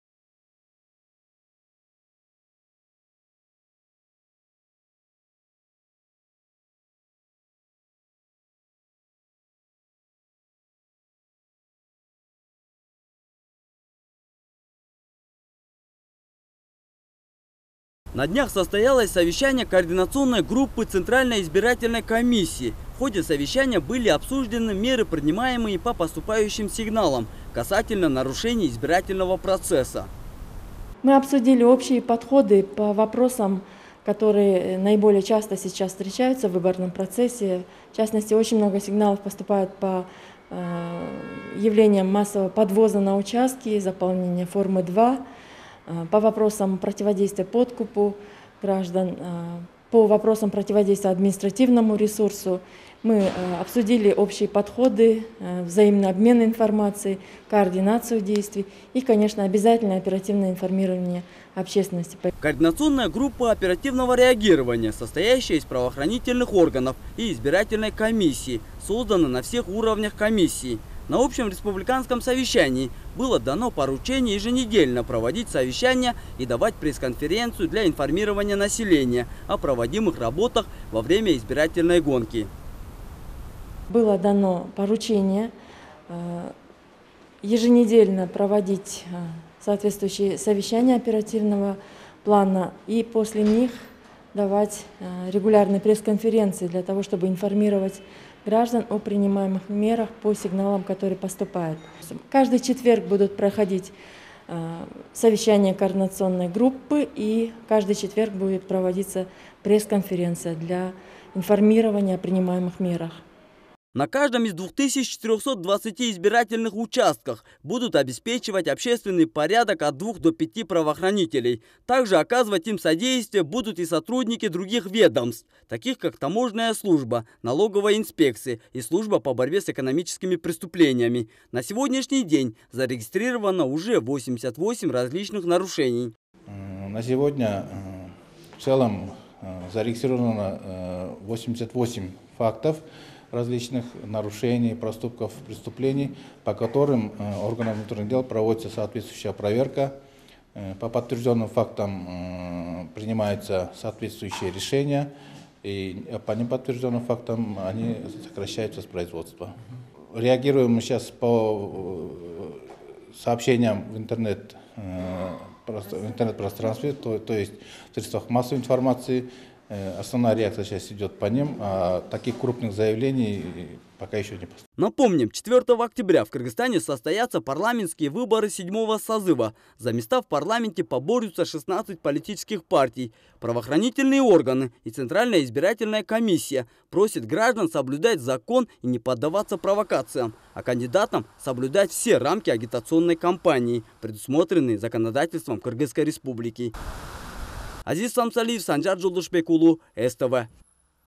Состоялось совещание координационной группы, Центральной избирательной комиссии КР
ТЕМА: НЕ ПРОДАВАЙ ГОЛОС И УВАЖАЙ ВЫБОР ДРУГИХ
На днях, Состоялось совещание координационной группы, Центральной избирательной комиссии. В ходе совещания были обсуждены, меры принимаемые по поступающим сигналам, касательно нарушений избирательного процесса.